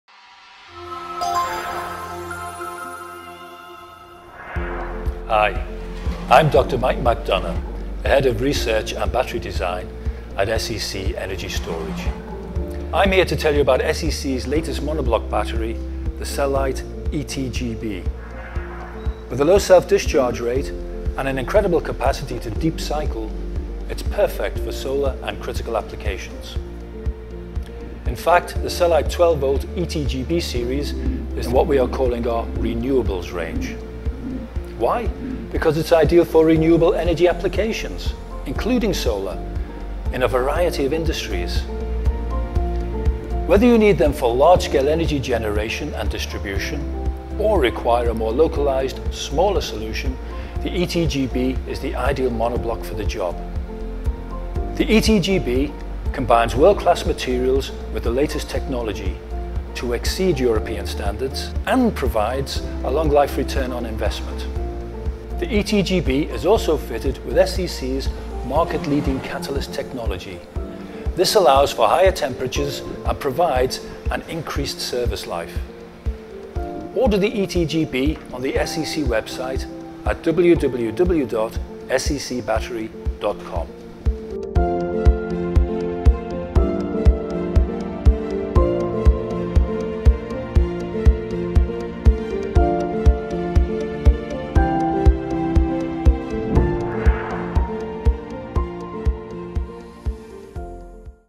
Introducing the CELLYTE ETGB a powerful 12Volt OpzV monobloc from SEC Battery
SEC is taking the renewables market by storm with our latest 12V tubular plate OpzV monobloc. Discover the ETGB in this short interview with SEC’s head of battery design, Dr Michael McDonagh.